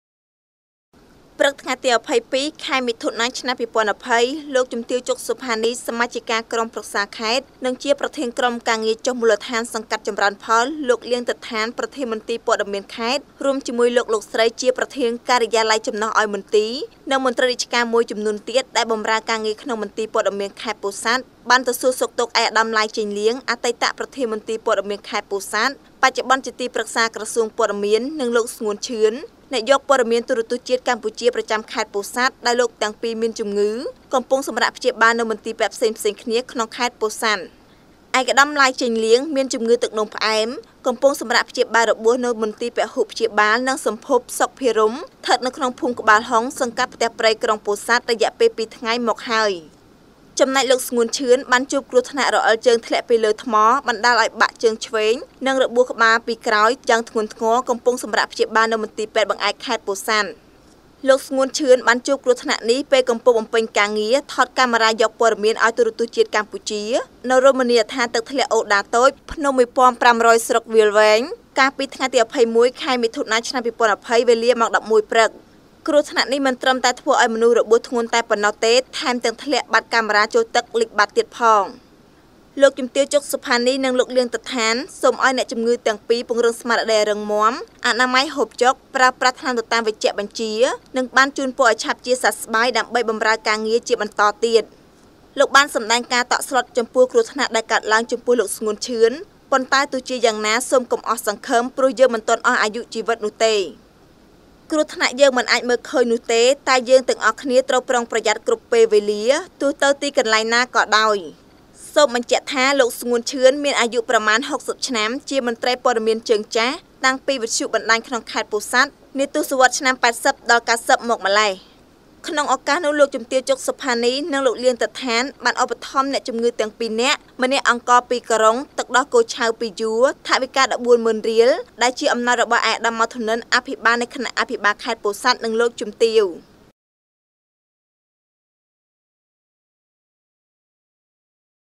លោកជំទាវ ជុក សុផានី និងលោក លាង ទិតដ្ឋាន សួរសុខទុក្ខអ្នកជម្ងឺកំពុងសម្រាកព្យាបាលនៅមន្ទីរពេទ្យ
ព្រឹកថ្ងៃទី២២ខែមិថុនាឆ្នាំ២០២០ លោកជំទាវ ជុក សុផានី សមាជិកាក្រុមប្រឹក្សាខេត្ត និងជាប្រធានក្រុមការងារចុះមូលដ្ឋានសង្កាត់ចំរើនផល  លោក លាង ទិដ្ឋាន ប្រធានមន្ទីរព័ត៌មានខេត្ត  រួមជាមួយលោក លោកស្រី ជាប្រធានការិយាល័យចំណុះឲ្យមន្ទីរ និងមន្ត្រីរាជការមួយចំនួនទៀត ដែលបំរើការងារក្នុងមន្ទីរព៍ត៌មានខេត្តពោធិ៍សាត់ បានទៅសួរសុខទុក្ខឯកឧត្តម ឡាយ ចេងលាង អតីតប្រធានមន្ទីរព័ត៌មានខេត្តពោធិ៍សាត់ បច្ចុប្បន្នជាទីប្រឹក្សាក្រសួងព័ត៌មាន  និងលោក ស្ងួន ឈឿន អ្នកយកព័ត៌មានទូរទស្សន៍ជាតិកម្ពុជាប្រចាំខេត្តពោធិ៍សាត់ ដែលលោកទាំងពីរមានជម្ងឺ កំពុងសម្រាកព្យាបាលនៅមន្ទីរពេទ្យផ្សេងគ្នាក្នុងខេត្តពោធិ៍សាត់ ។
 ឯកឧត្តម ឡាយ ចេងលាង មានជម្ងឺទឹកនោមផ្អែម កំពុងសម្រាកព្យាបាលរបួសនៅមន្ទីរពហុព្យាបាលនិងសម្ភព សុខ ភីរម្យ ស្ថិតនៅភូមិក្បាលហុង សង្កាត់ផ្ទះព្រៃ ក្រុងពោធិ៍សាត់ រយៈពេលពីរថ្ងៃមកហើយ។ចំណែកលោក ស្ងួន ឈឿន បានជួបគ្រោះថ្នាក់រអិលជើងធ្លាក់ពីលើថ្ម បណ្ដាលឲ្យបាក់ជើងឆ្វេងនិងរបួសក្បាលពីក្រោយយ៉ាងធ្ងន់ធ្ងរ កំពុងសម្រាកព្យាបាលនៅមន្ទីរពេទ្យបង្អែកខេត្តពោធិ៍សាត់ ។ លោក ស្ងួន ឈឿន ជួបគ្រោះថ្នាក់នេះក្នុងពេលកំពុងបំពេញការងារ ថតកាមេរាយកព័ត៌មានឲ្យទូរទស្សន៍ជាតិកម្ពុជា នៅរមណីយដ្ឋានទឹកធ្លាក់អូរដាតូចភ្នំ១៥០០ស្រុកវាលវែង កាលពីថ្ងៃទី២១ខែមិថុនាឆ្នាំ២០២០វេលាម៉ោង១១ព្រឹក។ គ្រោះថ្នាក់នេះមិនត្រឹមតែធ្វើឲ្យមនុស្សរងរបួសធ្ងន់ធ្ងរប៉ុណ្ណោះទេ ថែមទាំងធ្លាក់កាមេរាចូលទឹកលិចបាត់ទៀតផង។
 លោកជំទាវ ជុក សុផានី និងលោក លាង ទិដ្ឋាន សូមឲ្យអ្នកជម្ងឺទាំងពីរពង្រឹងស្មារតីឲ្យរឹងមាំ អនាម័យ ហូបចុក ប្រើប្រាស់ថ្នាំទៅតាមវេជ្ជបញ្ជា និងបានជូនពរឲ្យឆាប់ជាសះស្បើយ ដើម្បីបានបម្រើការងារជាបន្តទៀត។ លោកបានសម្ដែងការតក់ស្លុតចំពោះគ្រោះថ្នាក់ដែលកើតឡើងចំពោះលោក ស្ងួន ឈឿន ប៉ុន្តែទោះយ៉ាងណាសូមកុំអស់សង្ឃឹម ព្រោះយើងមិនទាន់អស់ជីវិតនោះទេ។ គ្រោះថ្នាក់យើងមិនអាចមើលឃើញនោះទេ តែយើងទាំងអស់គ្នាត្រូវប្រុងប្រយ័ត្នគ្រប់ពេលវេលា ទោះទៅទីកន្លែងណាក៍ដោយ។ សូមបញ្ជាក់ថា លោក ស្ងួន ឈឿន មានអាយុប្រមាណ៦០ឆ្នាំ ជាអ្នកយកព័ត៌មានជើងយូរ តាំងពីវិទ្យុបណ្ដាញក្នុងខេត្តពោធិ៍សាត់ នាទសវត្សឆ្នាំ៨០ ៩០មកម្ល៉េះ។
 ក្នុងឱកាសនោះលោកជំទាវ ជុក សុផានី និងលោក លាង ទិដ្ឋាដ្ឋាន បានឧបត្ថម្ភអ្នកជម្ងឺទាំង០២នាក់ ម្នាក់អង្ករ ០២ការ៉ុង ទឹកដោះគោឆៅ ០២យួរ ថវិការ ១៤ម៉ឺនរៀល ដែលជាអំណោយរបស់ឯកឧត្តម ម៉ៅ ធនិន អភិបាលនៃគណៈអភិបាលខេត្តពោធិ៍សាត់និងលោកជំទាវ៕ សែម សុគន្ធ